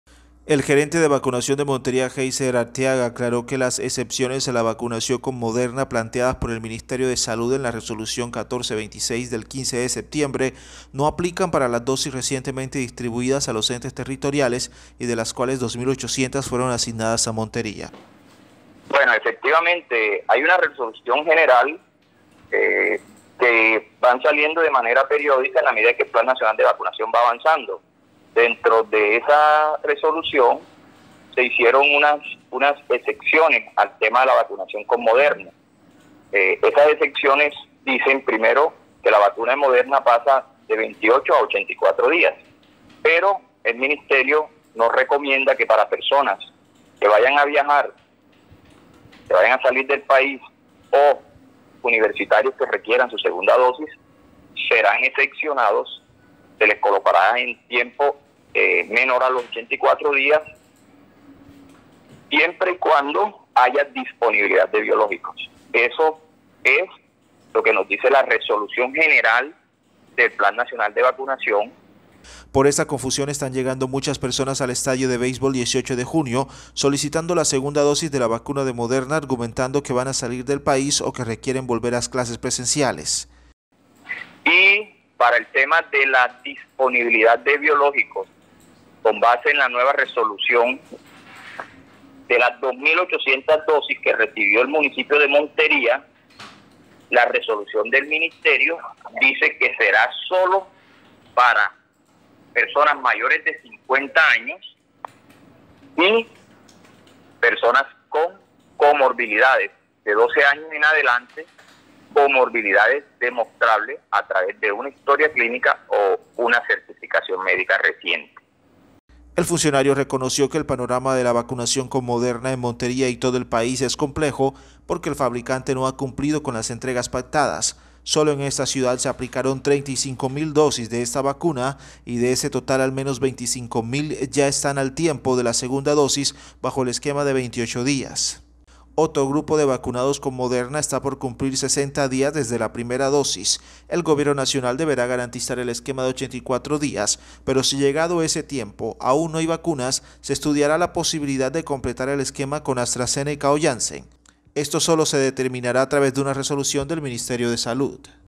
‘Excepciones a la vacuna de Moderna no aplican para estas 2.800 dosis’
‘Excepciones a la vacuna de Moderna no aplican para estas 2.800 dosis’: gerente de vacunación de Montería

Así lo aclaró el gerente de vacunación de Montería, Heiser Arteaga, al precisar que la reciente distribución de vacunas a la ciudad es exclusiva para población priorizada.

¡Siempre Informando! 📢/